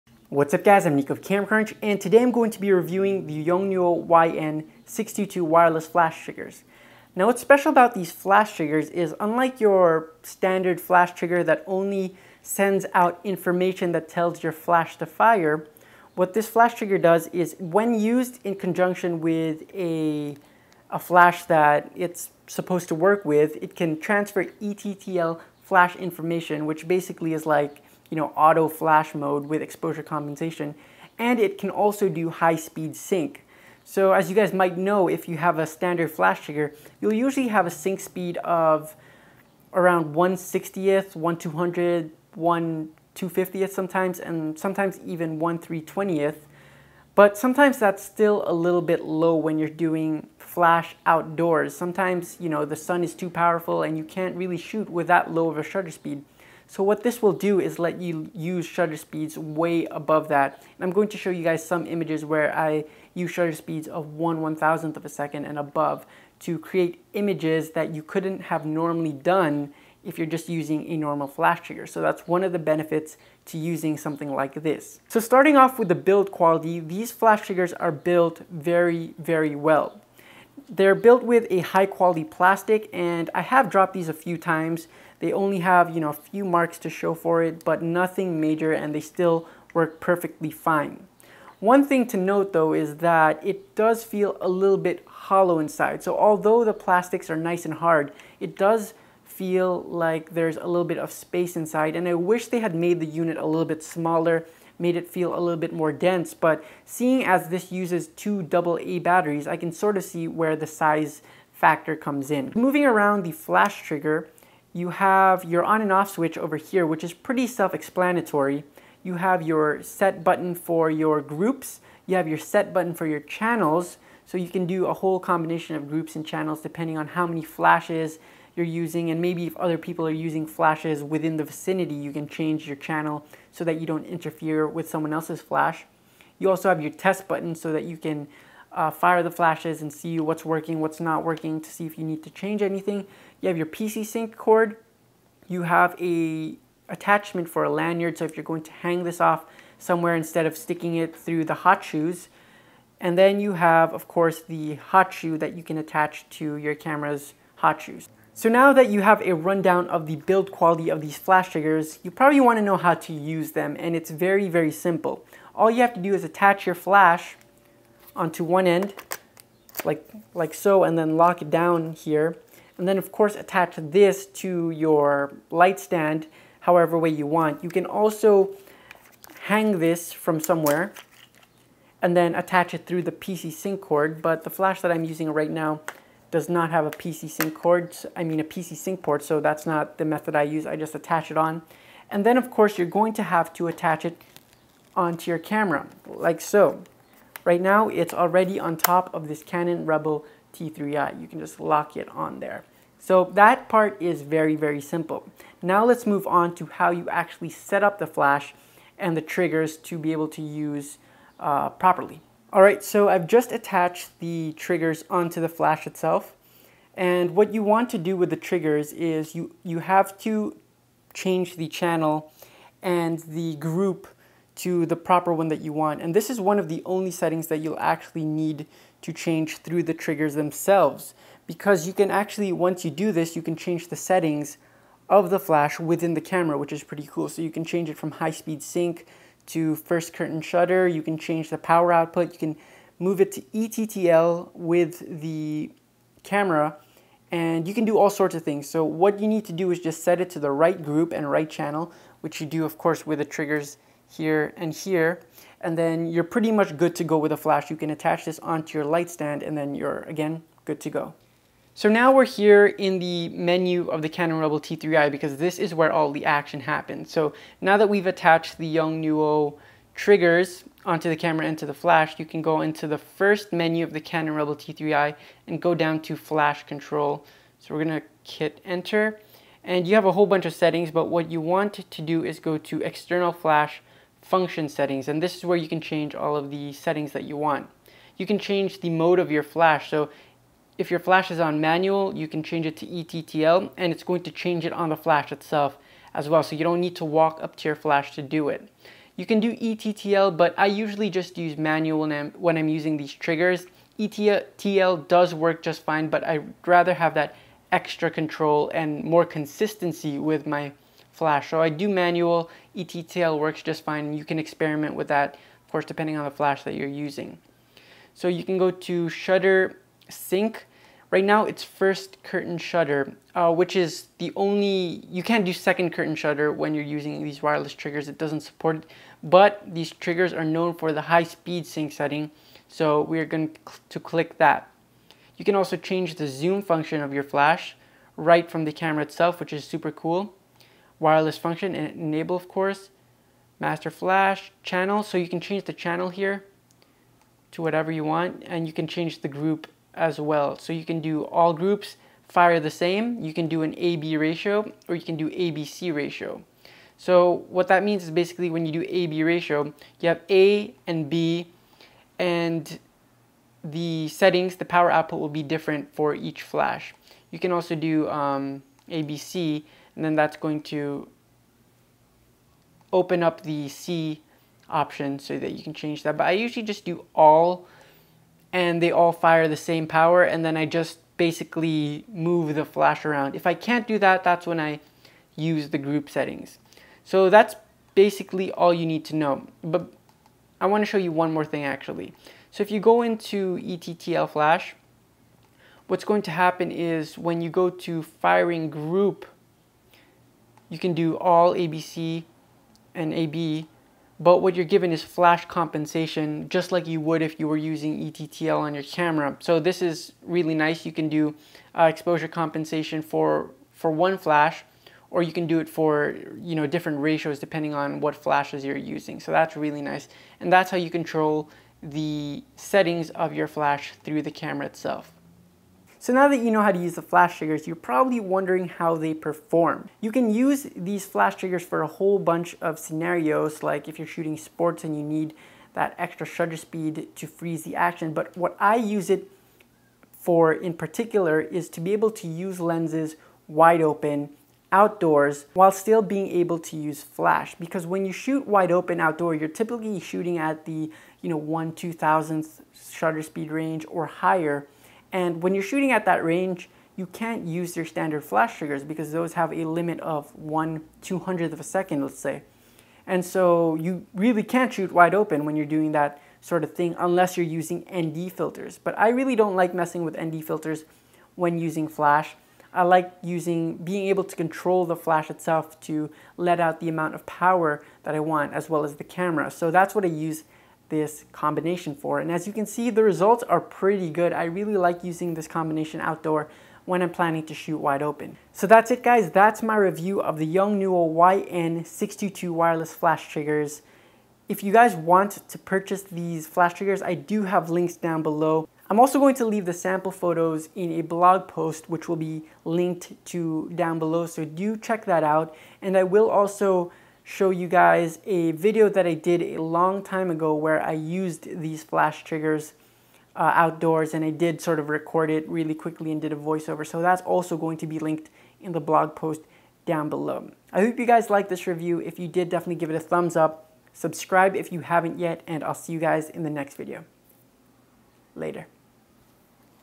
Yongnuo YN-622c Wireless Flash Triggers with High Speed Sync - Tutorial and Review
Yongnuo YN622c Wireless Flash Triggers with High Speed Sync - Tutorial and Review

Yonguo YN622c (Canon)
Yonguo YN622n (Nikon)

Compatible Flashes

Yonguo YN-568ex (Canon)
Yonguo YN-568ex (Nikon)

Try Amazon Prime for free and help support us

My Equipment